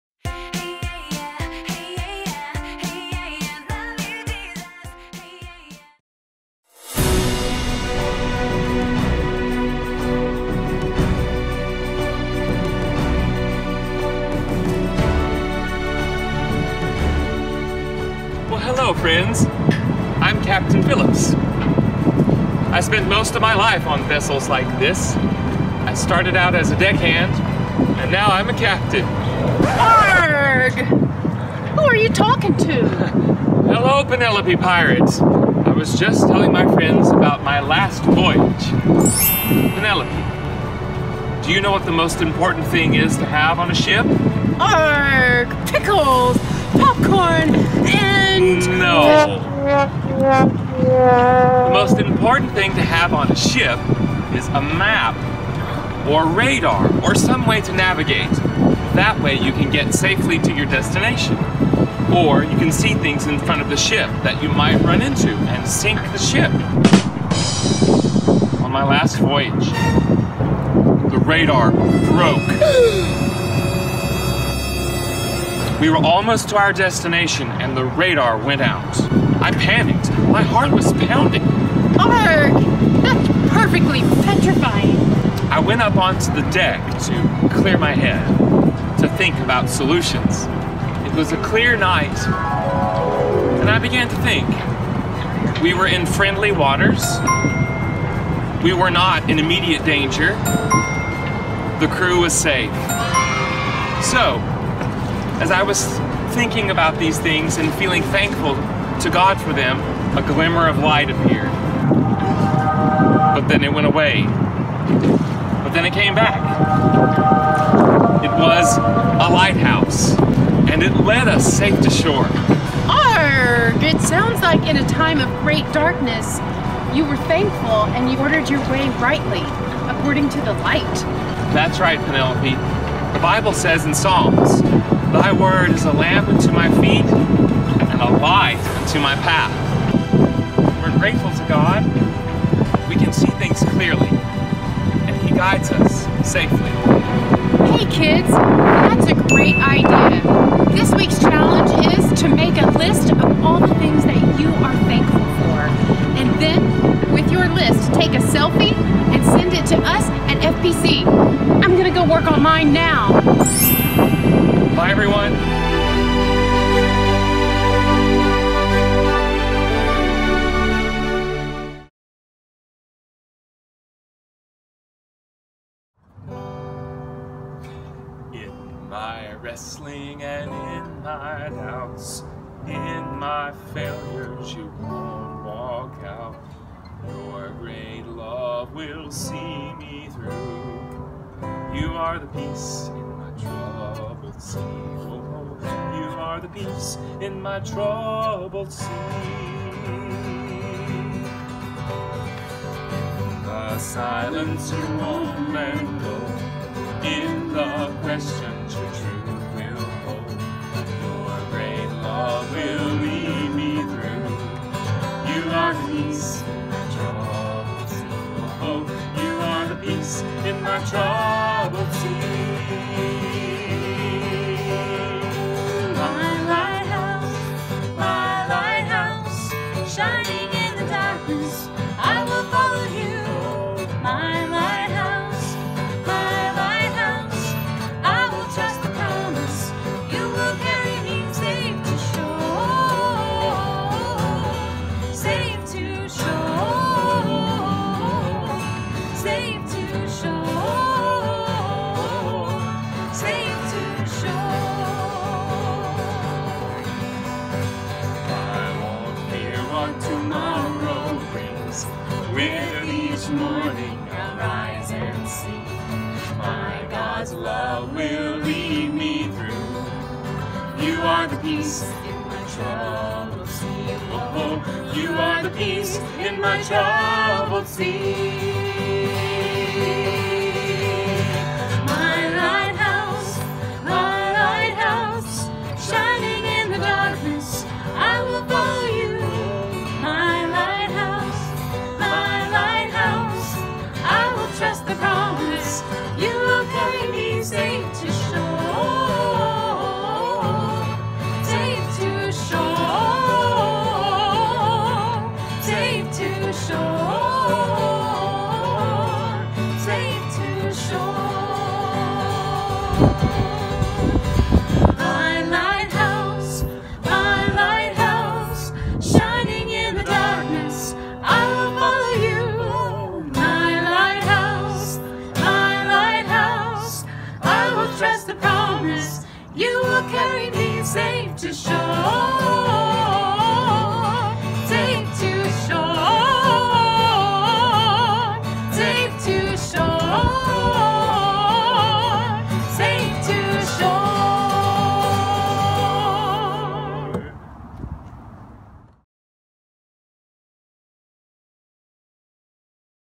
Week 3 Thankfulness
Introducing Captain Phillips, a totally cool sea captain! Captain Phillips regales us with a tale of the tempestuous sea when the radar on his ship suddenly stops working. How will he ever lead his crew safe to shore? Discover, along with Penelope Pirate, how Captain Phillips deals with this huge problem.

This week’s theme is thankfulness. During life’s journey, we often lose our way. Being thankful during times of great darkness can help order our mindset back to God and his Word. So, this week’s challenge has children making a list of all the things they are thankful for, taking a selfie with their list, and sending it to . Don’t miss out!